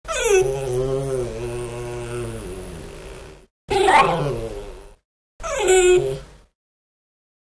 the Astro Puppy-Jump
here is the 3-month-old's jump attack with his whining thrown in for audio